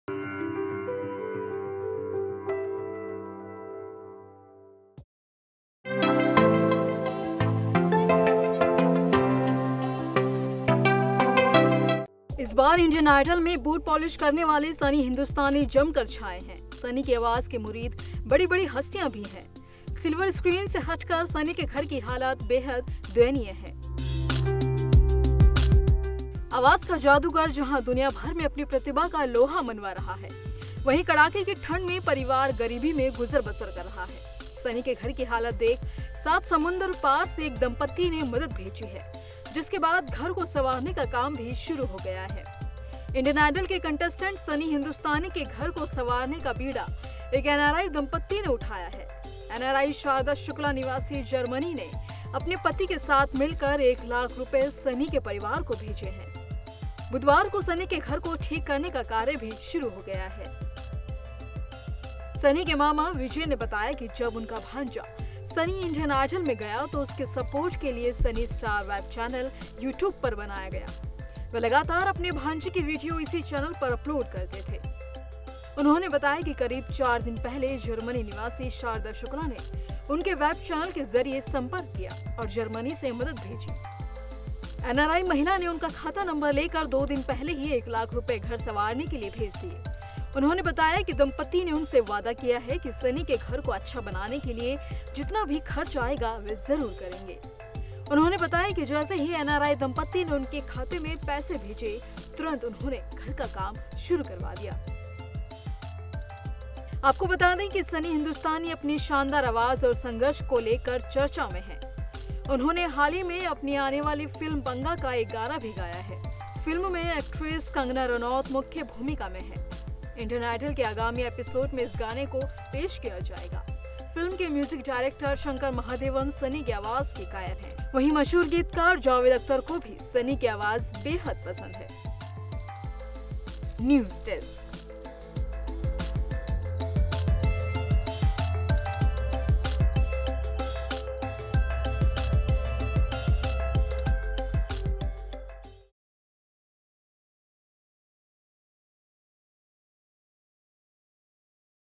Bathinda: Sunny Hindustani की आवाज के मुरीद हैं ये दंपती, घर की हालत देखी तो Germany से भेजे पैसे
इस बार इंडियन आइडल में बूट पोलिश करने वाले सनी हिंदुस्तानी जमकर छाए। सनी की आवाज की मुरीद बड़ी-बड़ी हस्तियां भी हैं। सिल्वर स्क्रीन से हटकर सनी के घर की हालत बेहद दयनीय है। आवाज का जादूगर जहां दुनिया भर में अपनी प्रतिभा का लोहा मनवा रहा है। वहीं कड़ाके की ठंड में परिवार गरीबी में गुजर बसर कर रहा है। सनी के घर की हालत देख सात समंदर पार से एक दंपती ने मदद भेजी है। जिसके बाद घर को संवारने का काम शुरू हो गया है।